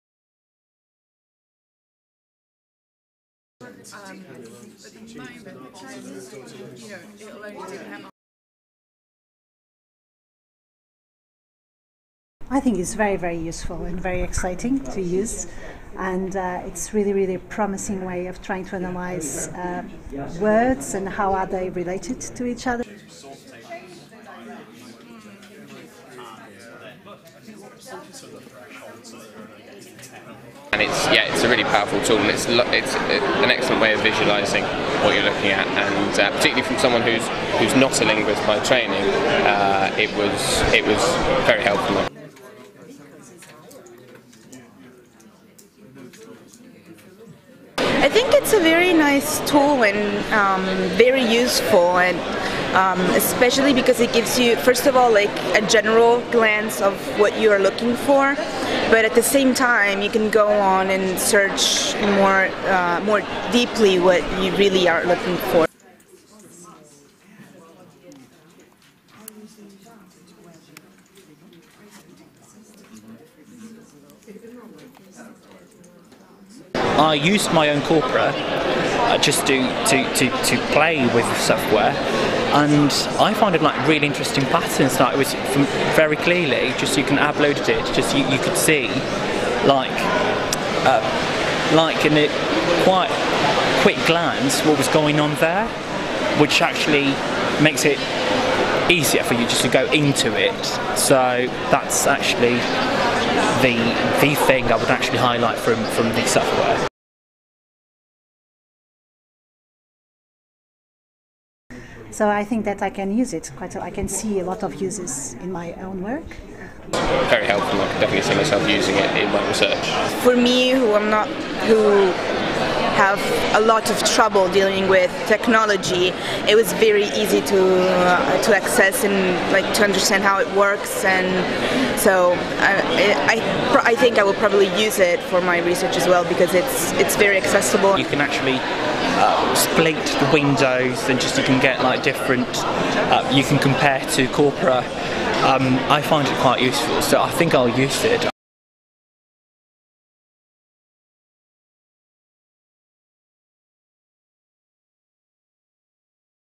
lancsbox
A new desktop corpus tool from Lancaster University - first impressions from the tool.